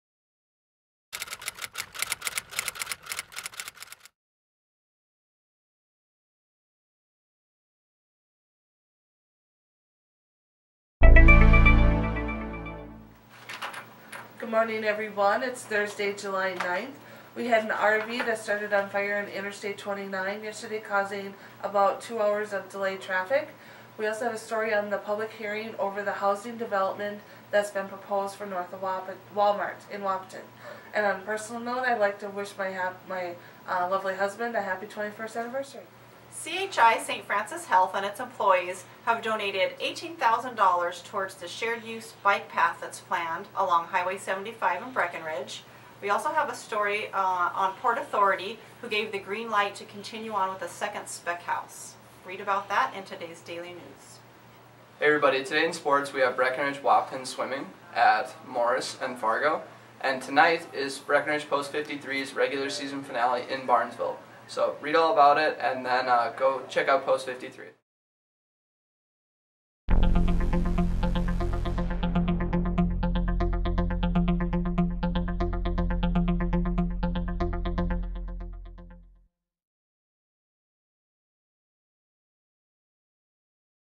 Hot Off the Press - RV fire, Wahpeton development opposition
A preview of stories in today's Daily News - RV catches fire in I29, Wahpeton City Council hears opposition to planned housing development, Breckenridge gives green light for second spec house, BW swim team